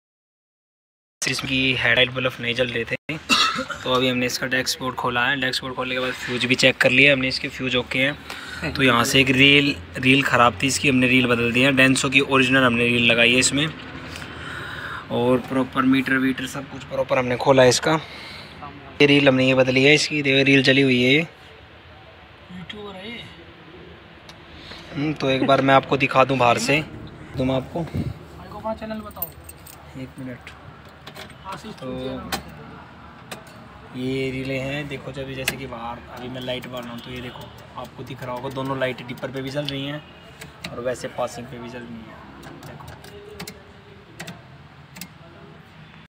Ager head light ka facus chahiye toh ye lga वाए।
thxx for watching 👀👀

I hope enjoy the video🎥😃❤

Tata ace gold wiring problem
Full wire check Tata ace gold bs6 petrol